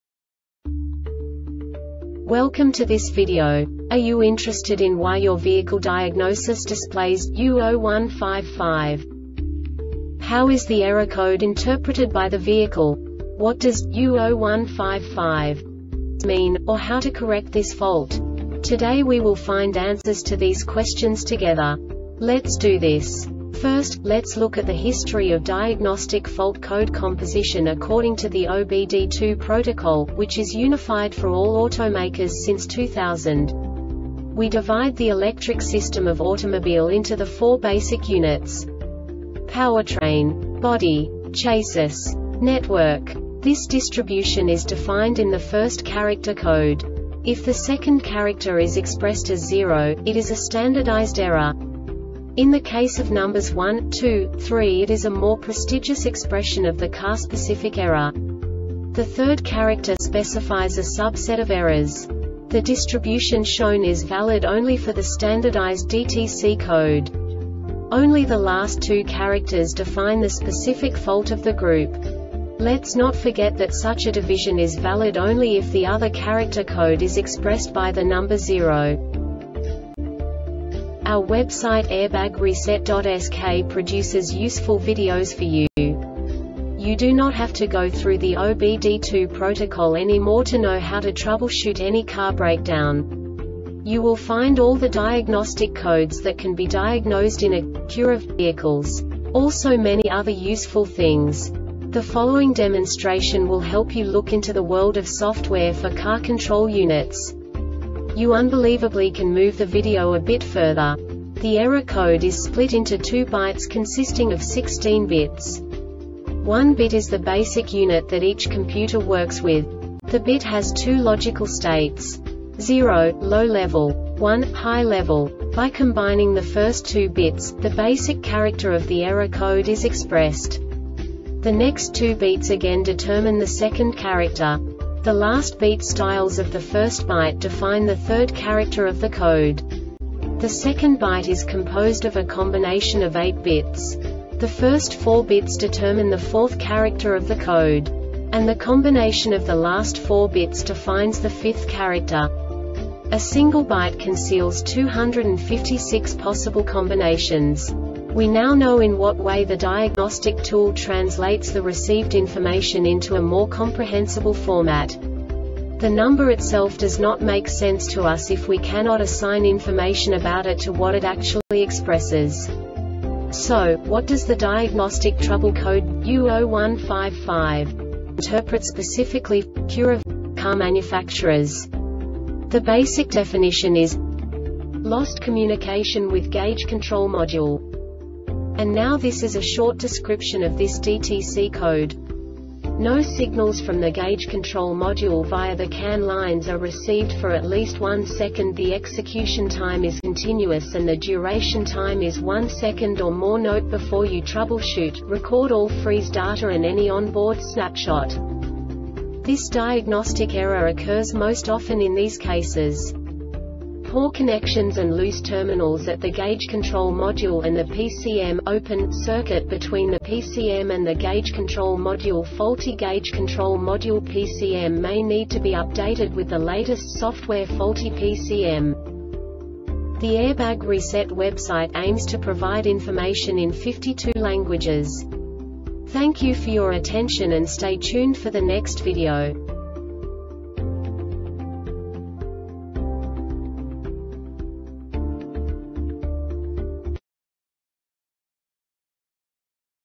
DTC Acura U0155 Short Explanation
Make:
Acura
Code:
U0155
Definition:
Lost Communication with Gauge Control Module
Description:
No signals from the gauge control module via the CAN lines are received for at least 1 second. The execution time is continuous and the duration time is 1 second or more. NOTE: Before you troubleshoot, record all freeze data and any on-board snapshot.
Cause:
Poor connections and loose terminals at the gauge control module and the PCM
"Open" circuit between the PCM and the gauge control module
Faulty Gauge Control Module
PCM may need to be updated with the latest software
Faulty PCM